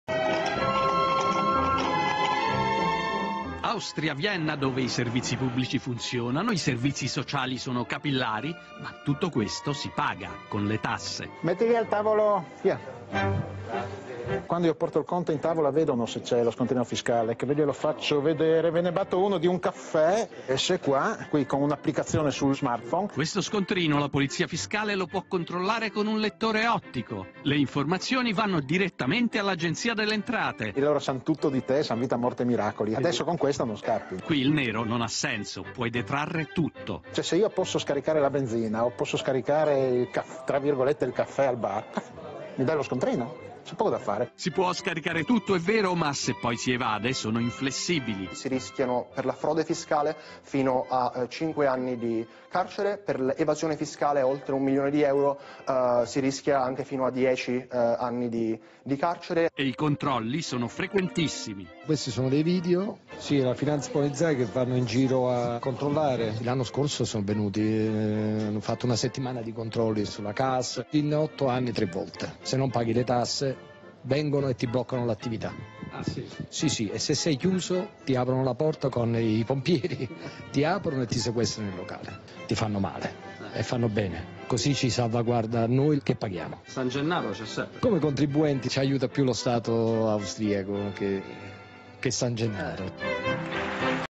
Fisco viennese
Scontrini digitalizzati, controlli della Finanzpolizei, pene severe per gli evasori. Così l'Austria fa pagare le tasse. Di Andrea Scazzola